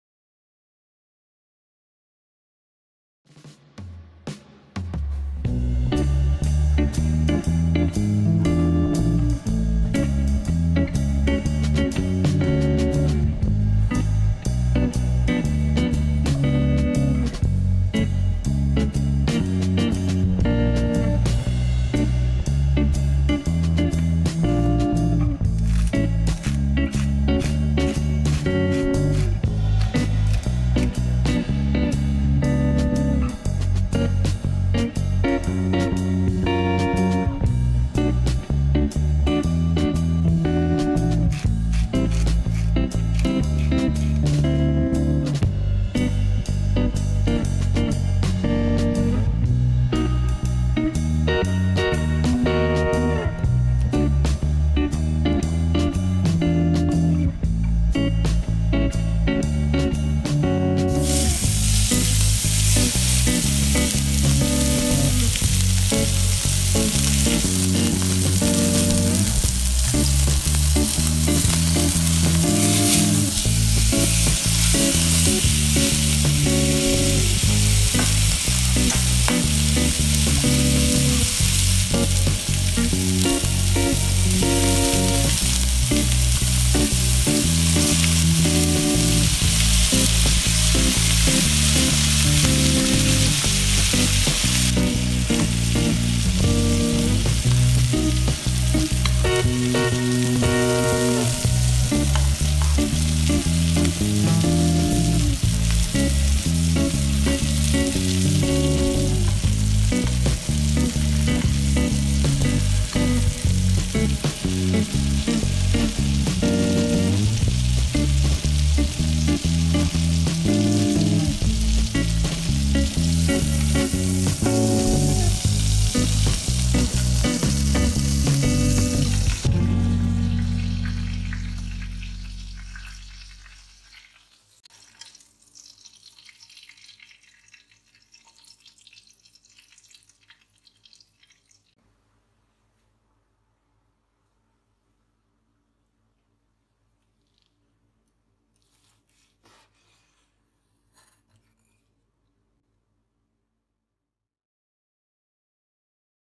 How to make Pan Seared STEAK - BUTTER Basted recipe
Ingredients:
Rib-eye
Rosemary
Garlic cloves
Butter
Garlic powder